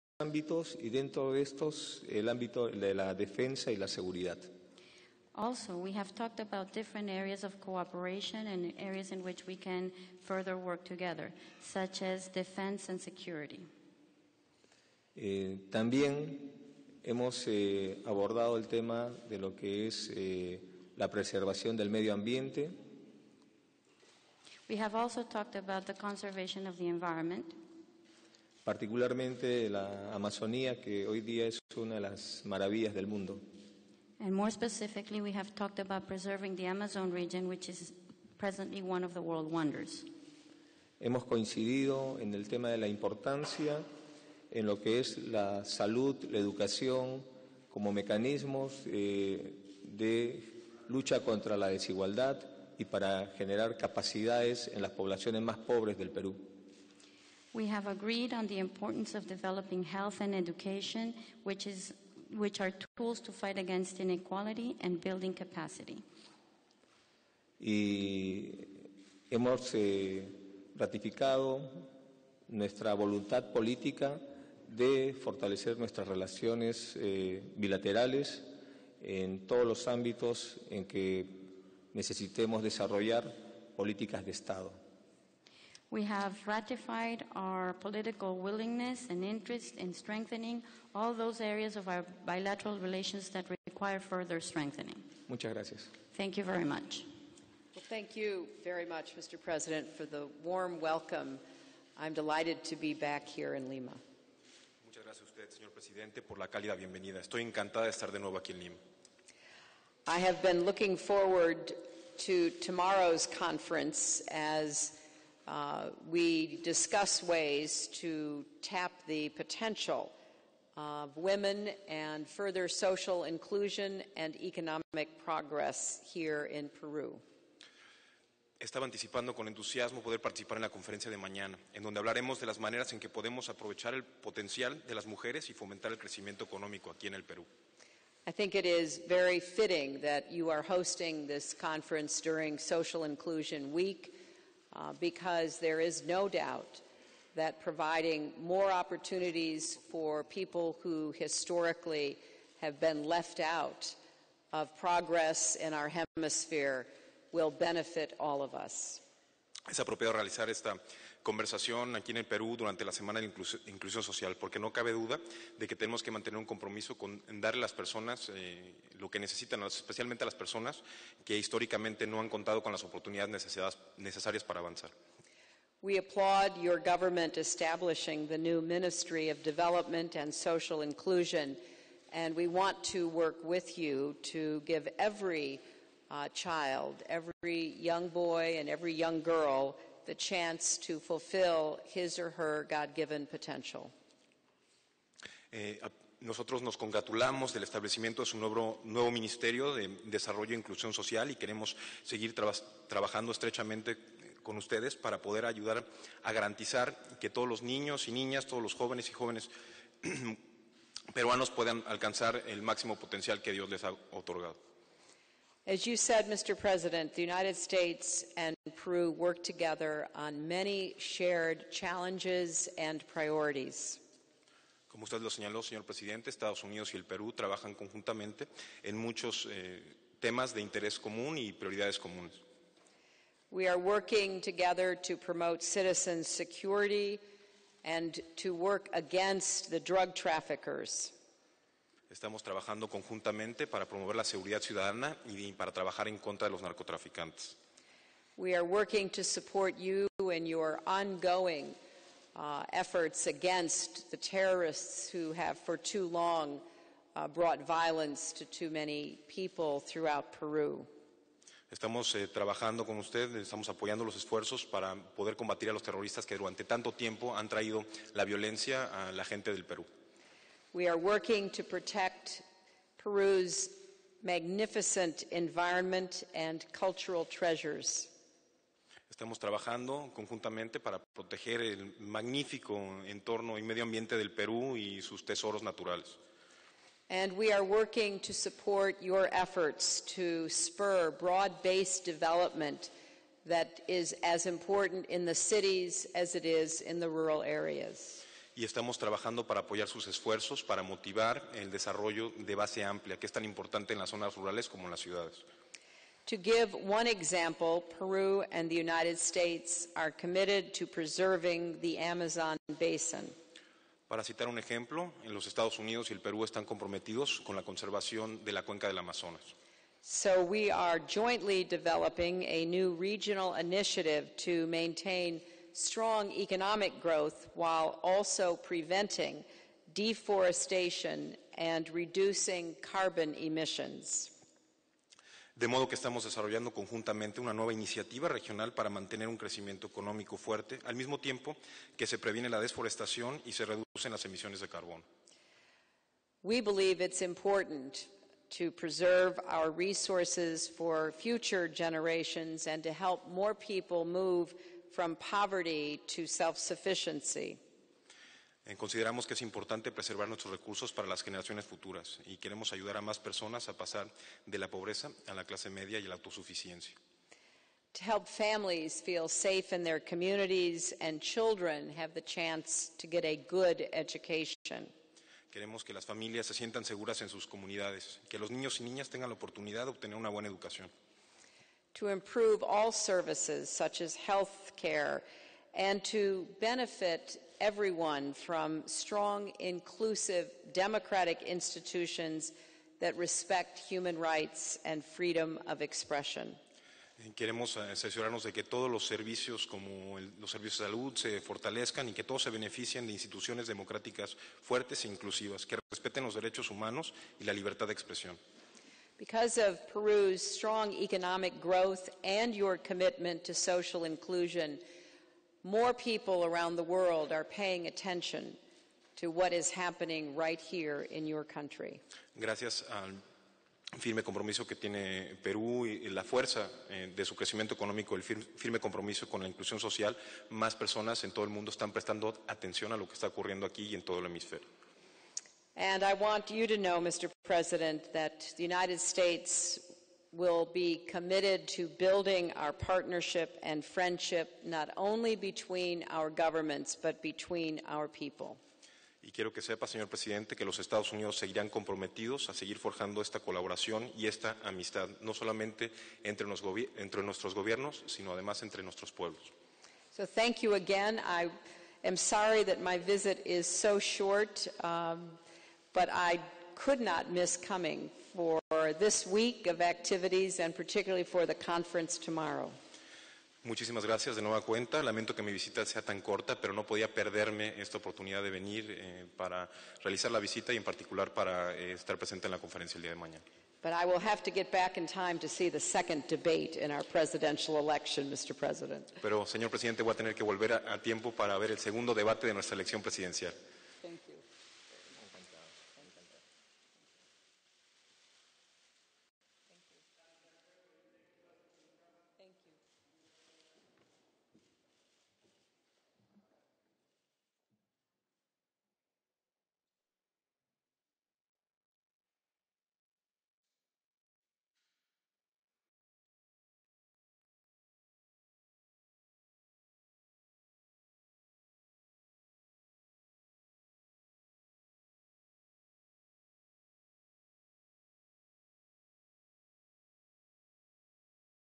Mandatario recibió en audiencia a Hillary Clinton, Secretaria de Estado de los Estados Unidos II
Jefe de Estado recibió en audiencia a Hillary Clinton, Secretaria de Estado de los Estados Unidos